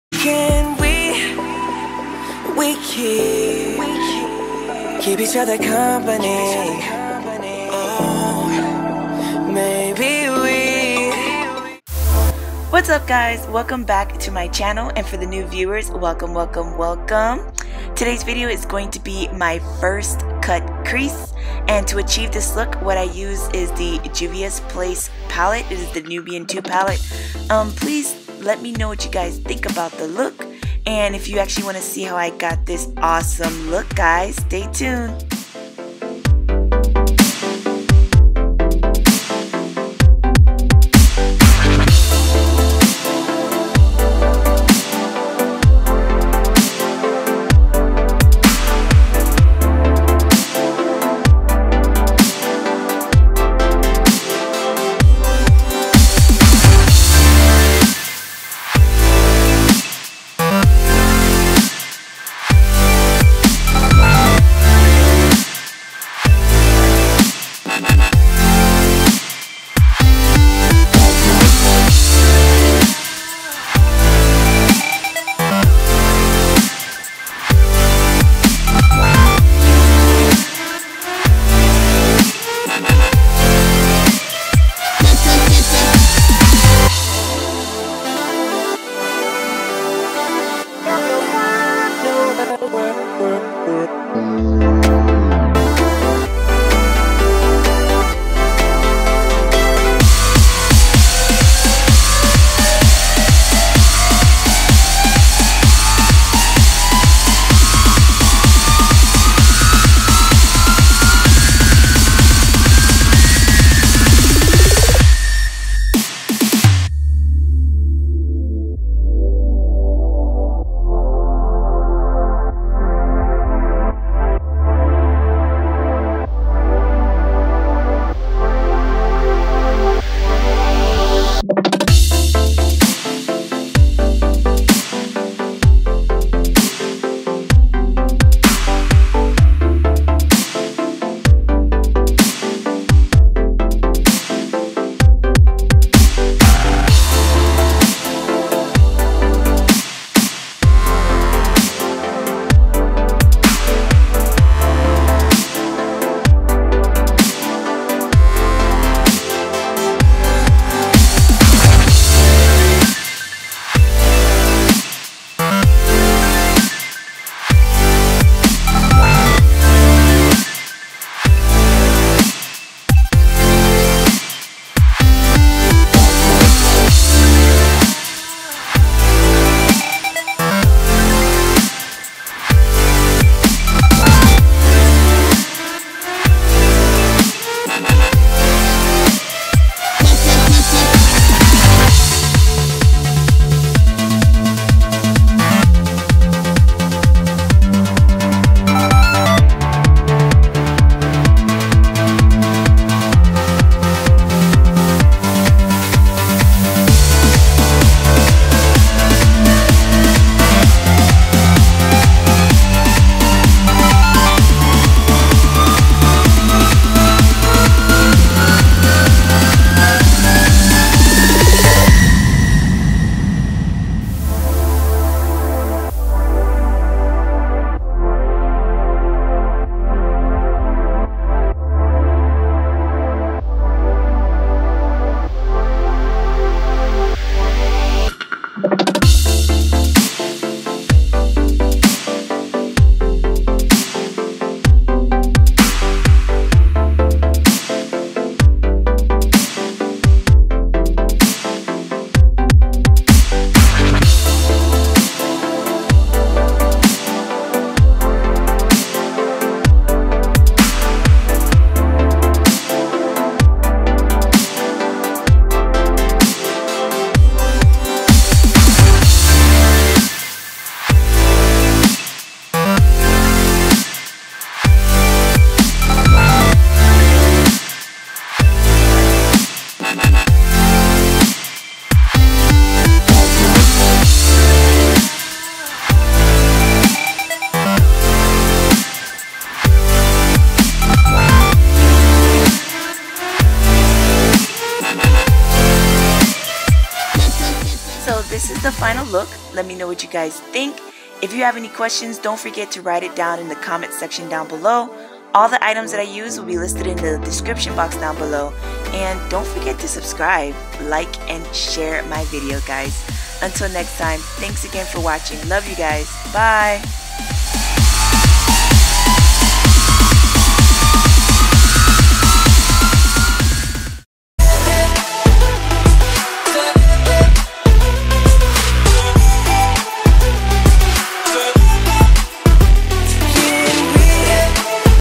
My First Cut Crease
Items Used:
Juvia's Place Nubian 2 palette
Beauty Bakerie Black Milk Gelato
Urban Decay Heavy Metal Glitter Eyeliner

Camera Used:
Sony a5100